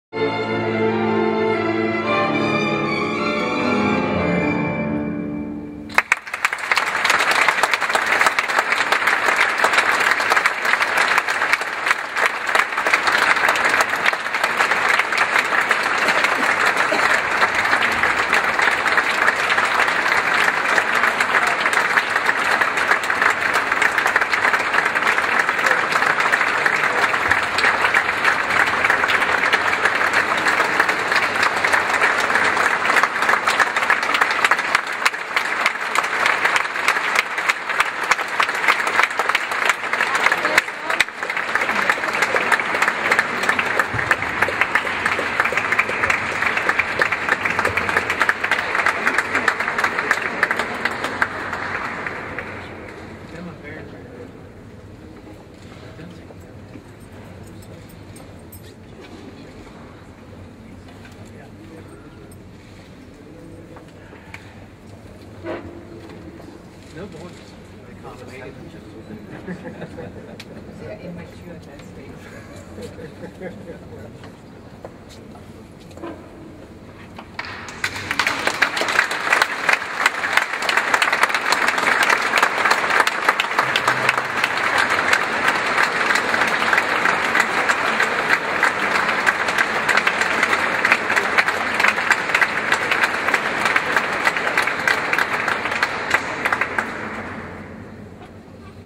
Presidents Napolitano and Christofias congratulating M. Kariolou and the CY Young Strings Soloists
Concert at Quirinale Palace Rome (Palazzo del Quirinale Roma)performed by the "Cyprus Young Strings Soloists" (10-12 years old), dir. Prof. Matheos Kariolou.
Concert in the presence of the President of Italy, Mr. Giorgio
Napolitano and of the President of Cyprus, Mr. Demetris Christofias, on the occasion of the Cyprus Presidency of the European Union.
LIVE from the Palazzo Quirinale Rome, Oct. 17th, 2012